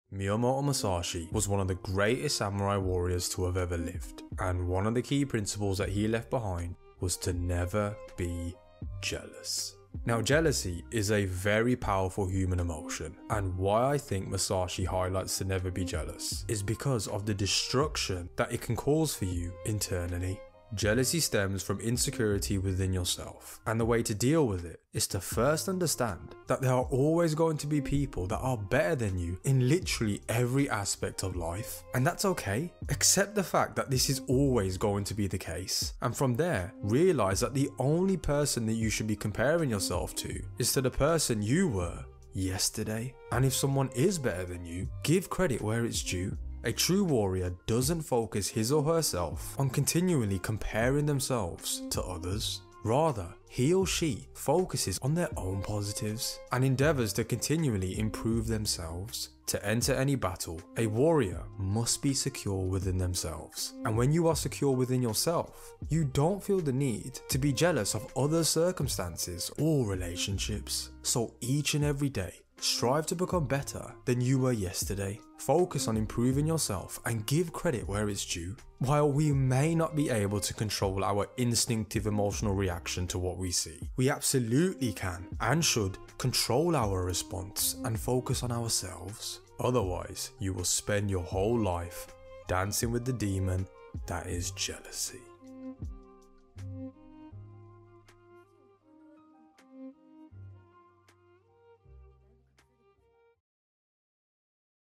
Never Be Jealous
Miyamoto Musashi was one of the greatest samurai warriors to of ever lived and one of the key principles he left behind was to “Never be jealous”

Now jealousy is a very powerful human emotion and why I think Musashi highlights to never be jealous is because of the destruction that it can cause for you internally.

Jealousy stems from insecurity within yourself - and the way to deal with it
is to first understand there are always going to be people that are better than you in literally every aspect of life - and that’s ok!

Accept the fact that this is always going to be the case - and from there realise that the only person you should be comparing yourself to - is to the person you were yesterday.

And If someone is better than you, give credit where it’s due.

A true warrior doesn’t focus his or herself on continually comparing themselves to others - rather he or she focuses on their own positives and endeavours to continually improve themselves.

To enter battle, a warrior must be secure within themselves - a within yourself, you don’t feel the need to be jealous of other’s circumstances or relationships.

So each and every day, strive to become better than you were yesterday - focus on improving yourself and give credit where it’s due.

While we may not be able to control our instinctive emotional reaction to what we see, we absolutely can and should control our response and focus on ourselves…otherwise you will spend your whole life dancing with the demon that is jealousy.